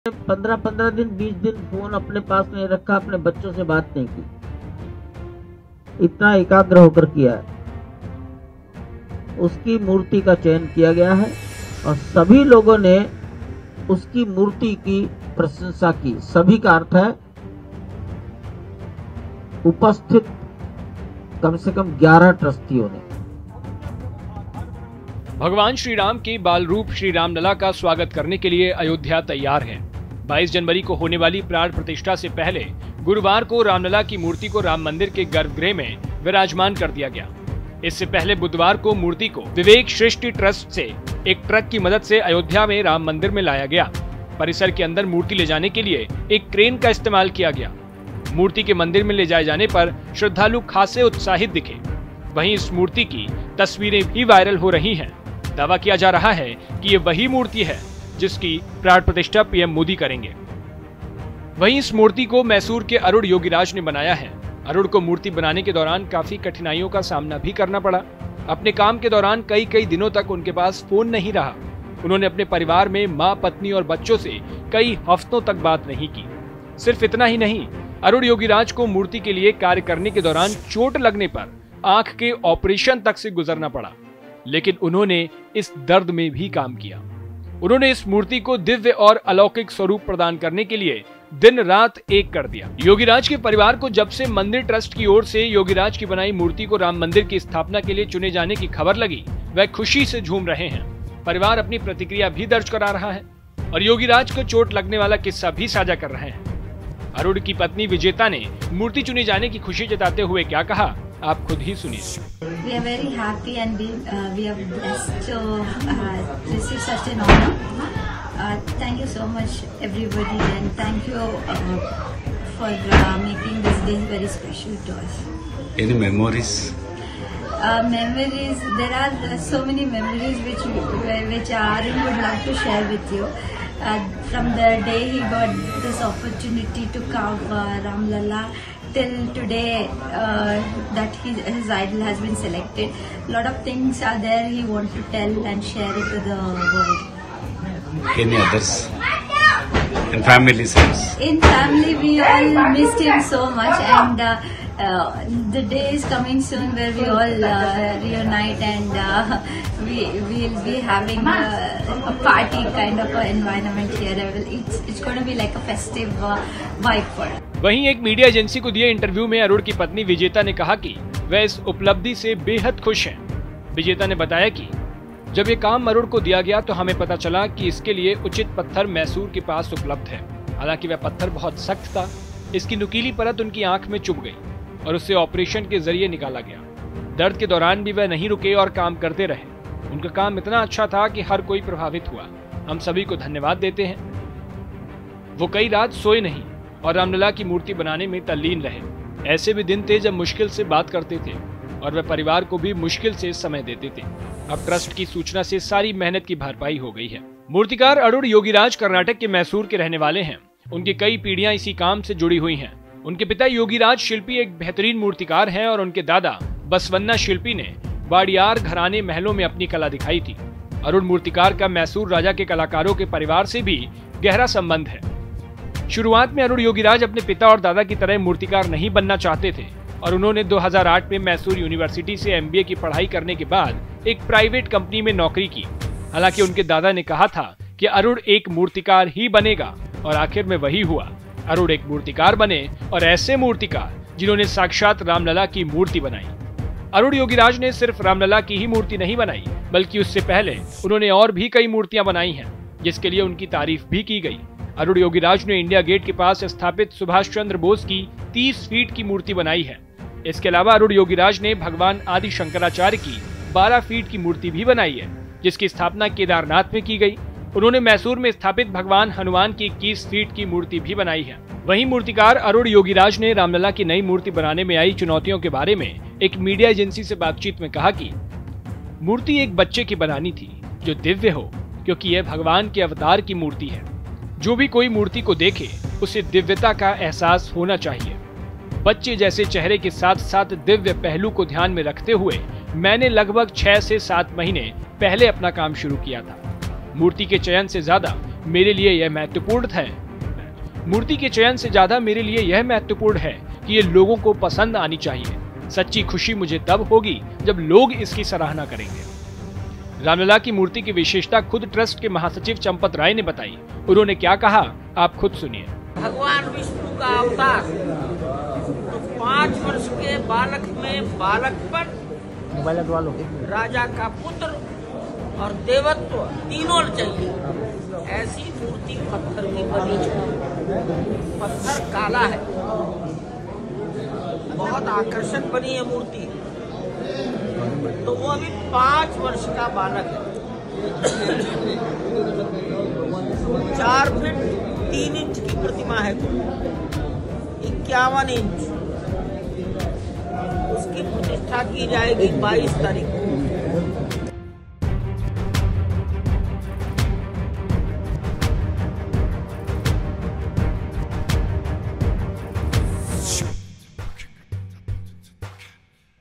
Ayodhya Ram Mandir: कौन हैं मूर्तिकार Arun Yogiraj जिनकी बनाई मूर्ति की होगी प्राणप्रतिष्ठा
भगवान श्रीराम के बालरूप श्री रामलला का स्वागत करने के लिए अयोध्या तैयार है. 22 जनवरी को होने वाली प्राण प्रतिष्ठा से पहले गुरुवार (18 जनवरी) को रामलला की मूर्ति को राम मंदिर के गर्भगृह में विराजमान किया जाएगा. इससे पहले बुधवार (17 जनवरी) को मूर्ति को विवेक सृष्टि ट्रस्ट से एक ट्रक की मदद से अयोध्या में राम मंदिर में लाया गया. परिसर के अंदर मूर्ति ले जाने के लिए क्रेन का इस्तेमाल किया गया. मूर्ति के मंदिर में ले जाए जाने पर श्रद्धालु खासे उत्साहित दिखे.

Producer -  Ujjwal
Editor- Ishika